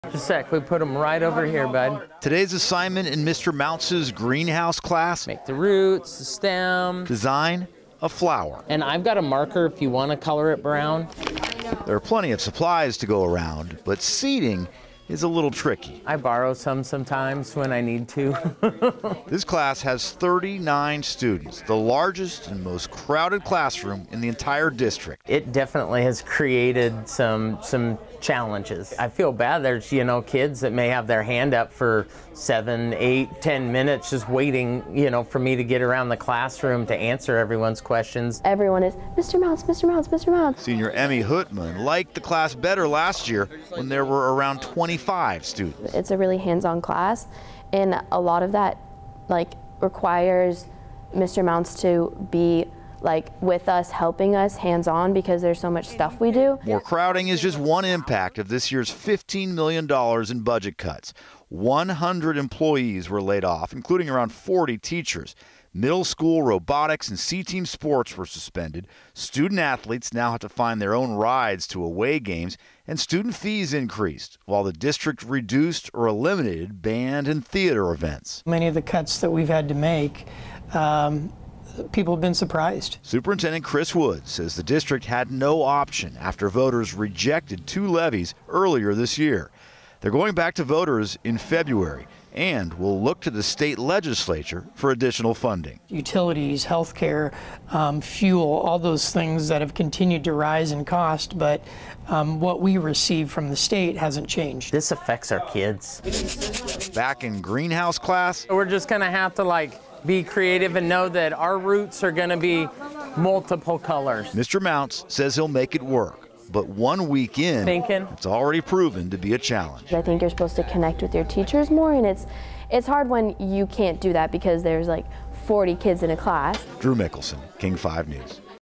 Yelm students feeling the impact of millions of dollars in budget cuts
A teacher with 39 students in one class describes limited time for addressing students' questions, lack of resources and more since the cuts.